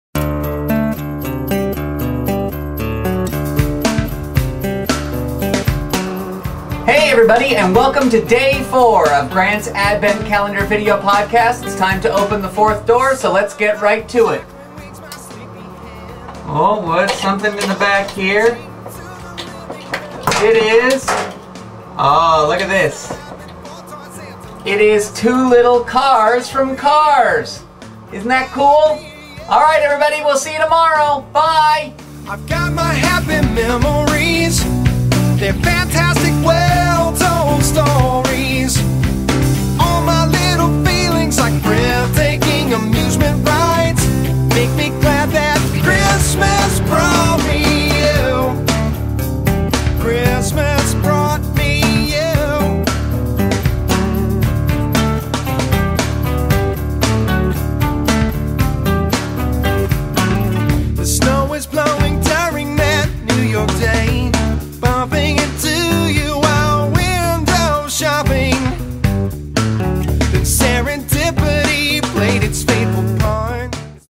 Grant's Advent Calendar 2008 Video Podcast - Day #004
Zoom in on Day #4!

A work safe and family friendly Video Podcast featuring the daily opening of the Advent Calendar Grant's Mom gave him!